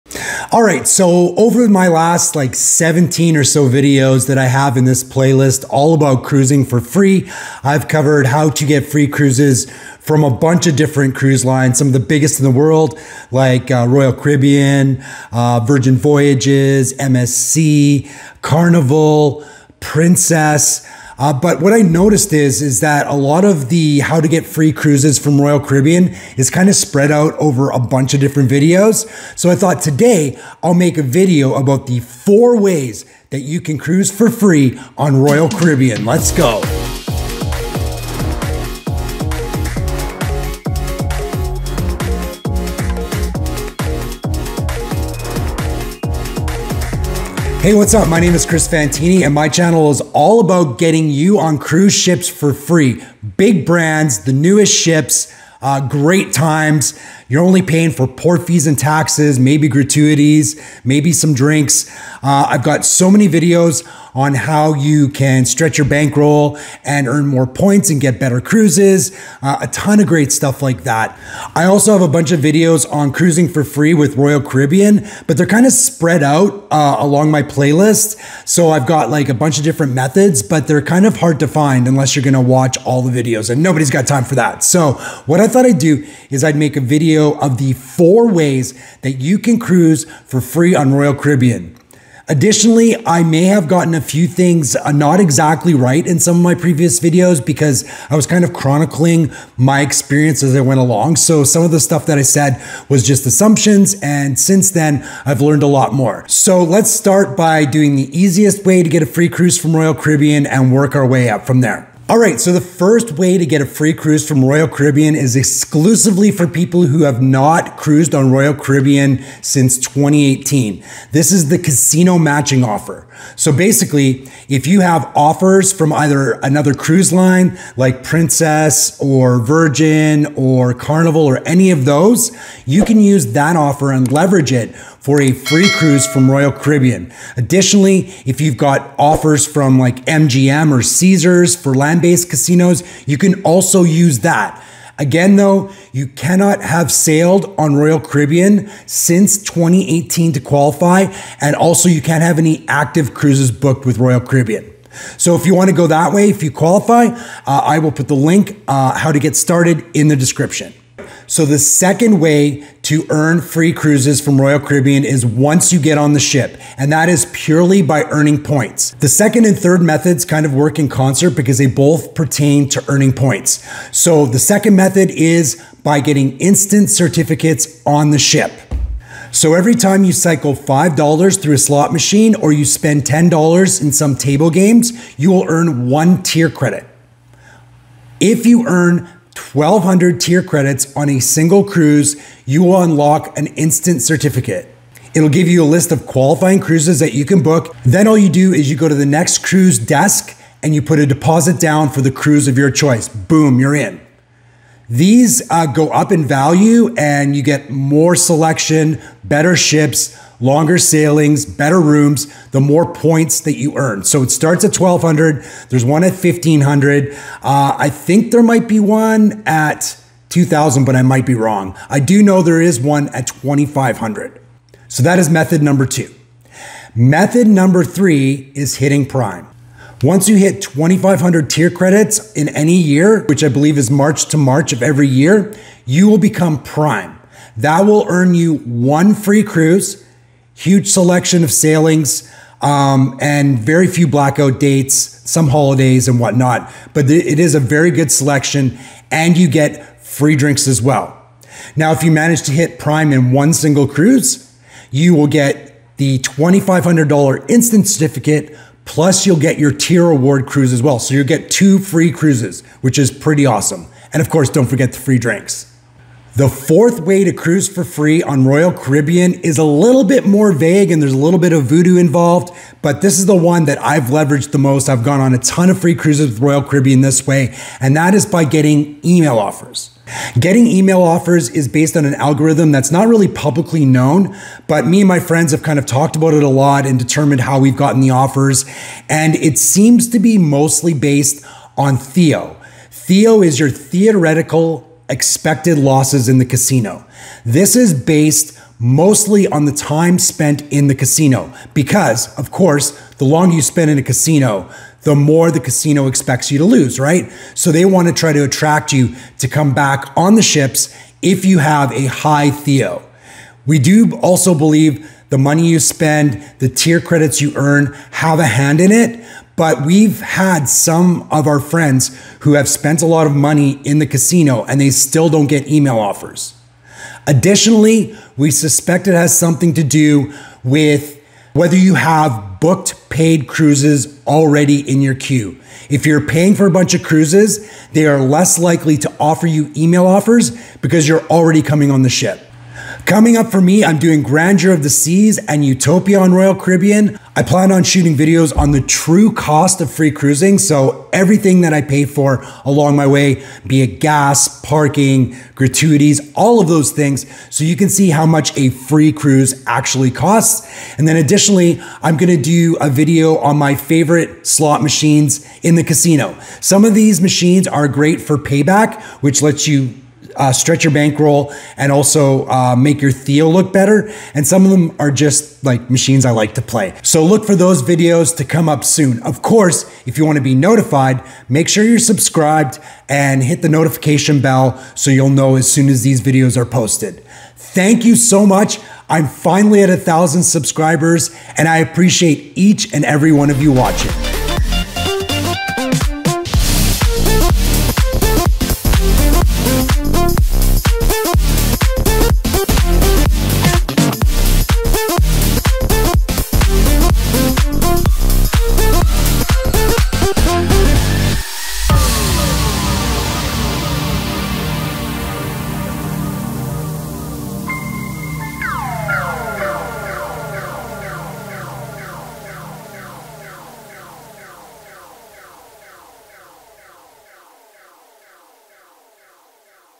4 Ways to Cruise for FREE on Royal Caribbean
I've talked A LOT about my comps on Royal Caribbean, but I thought it would be good to put all of the 4 main ways you can cruise for free with them in one quick video.

Why pay to cruise???